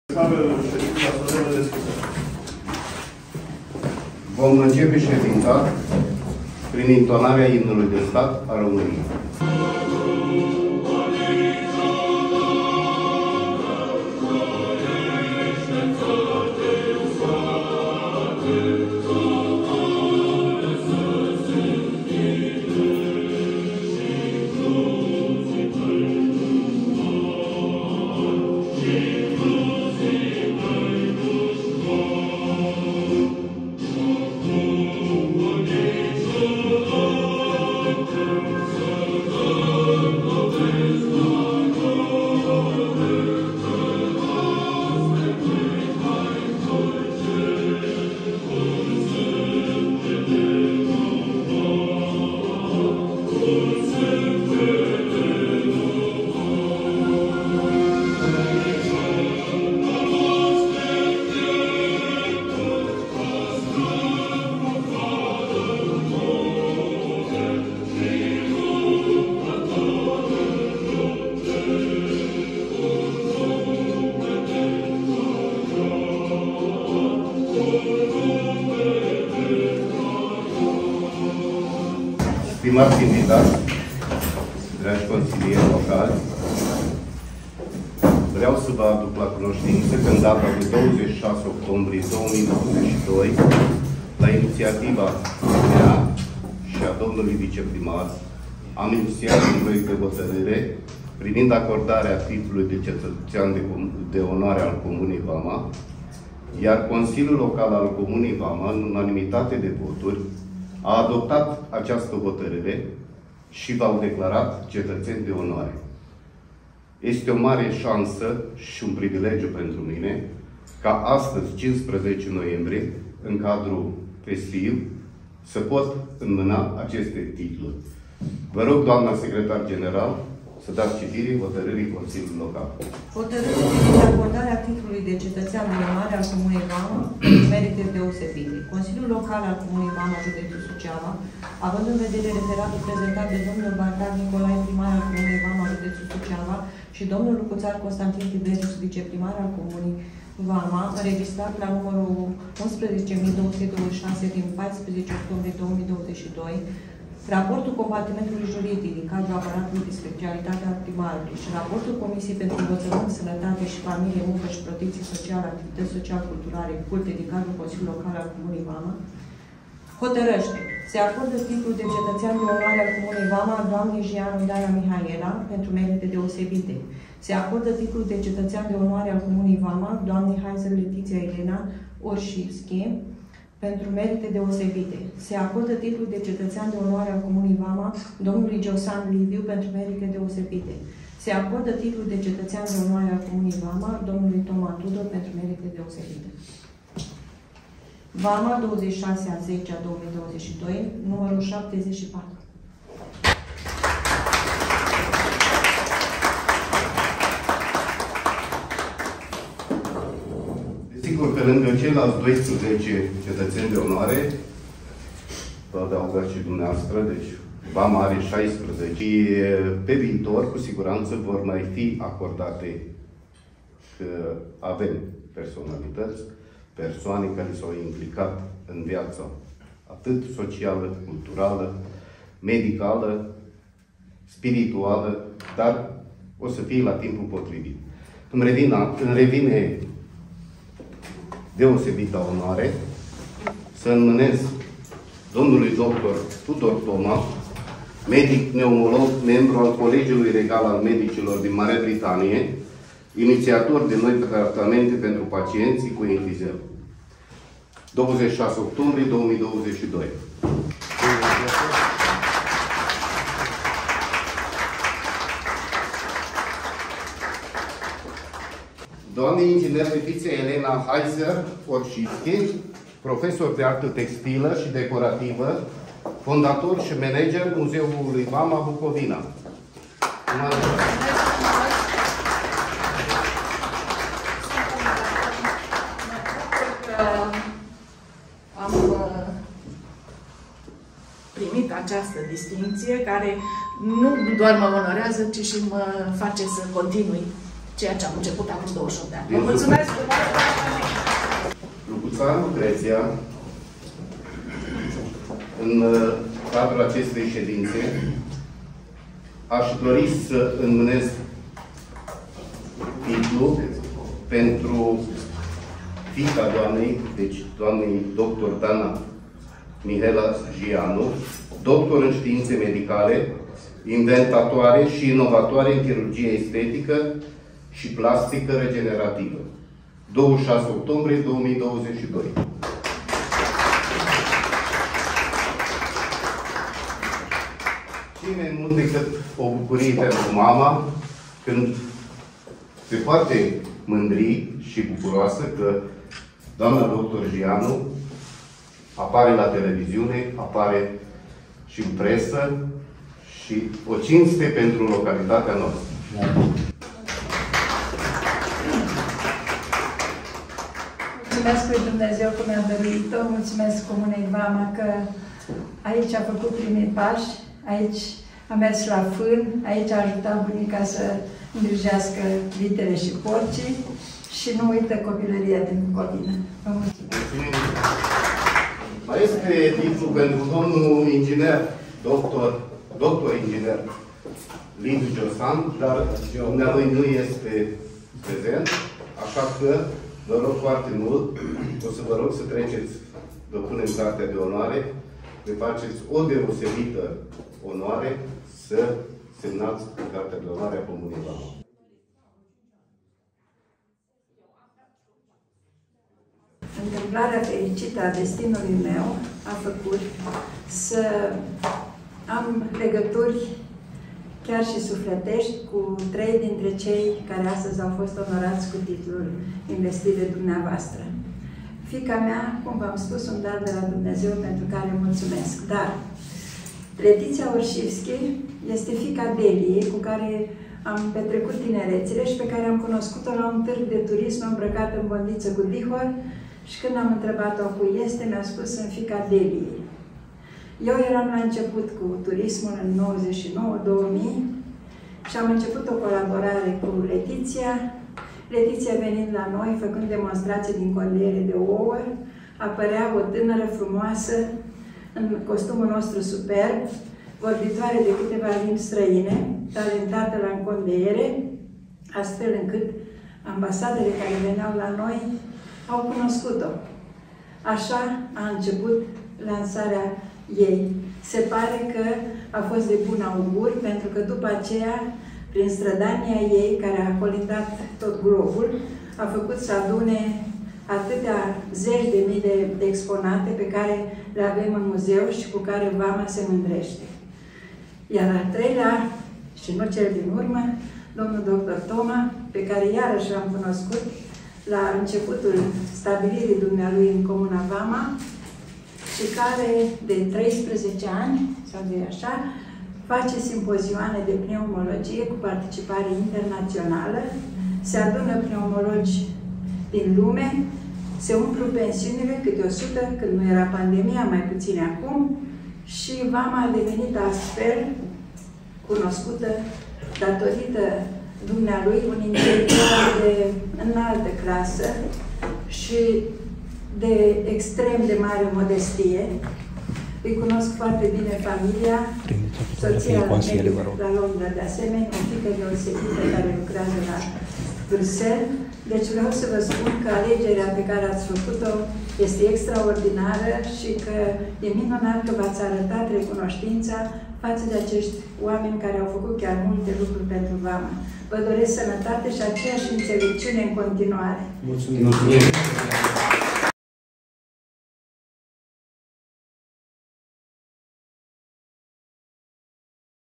A fost sedinta solemna la Primăria Vama. Autoritatile isi cinstesc valorile locale
Nu copiați și nu repostați!
Dacă vă place conținutul nostru, vă îndemnăm să îl distribuiți!